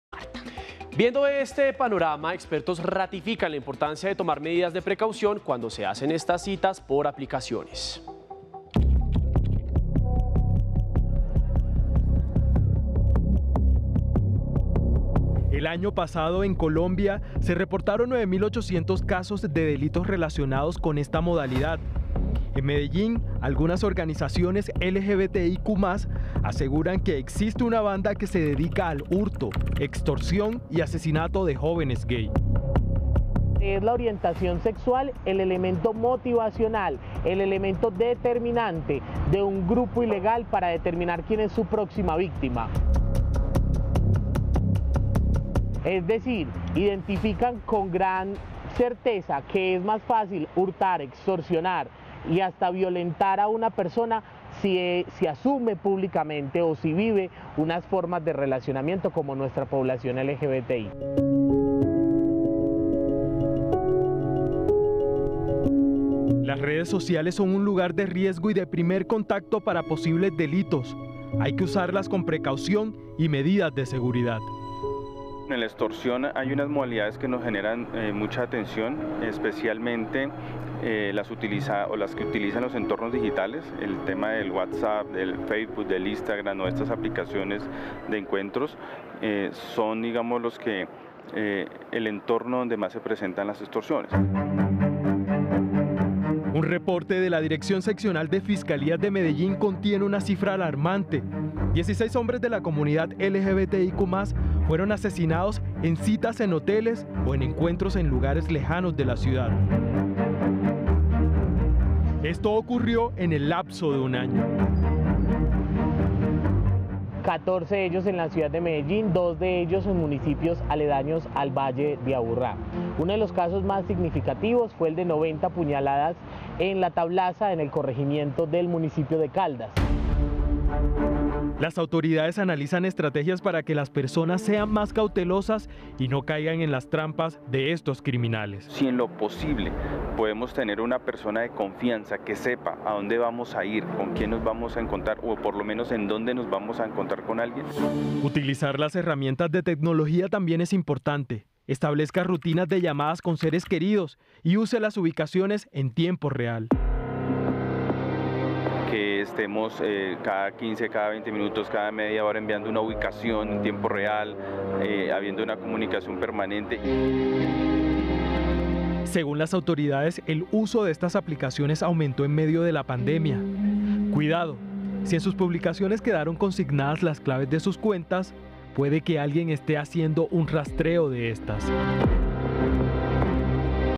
¿Qué medidas tomar para no ser víctima cuando se conoce a alguien por internet? | RTVC Noticias
Los investigadores del Gaula de la Policía Nacional, explican por qué al momento de buscar un amigo o una amiga por aplicación es indispensable tomar medidas de precaución.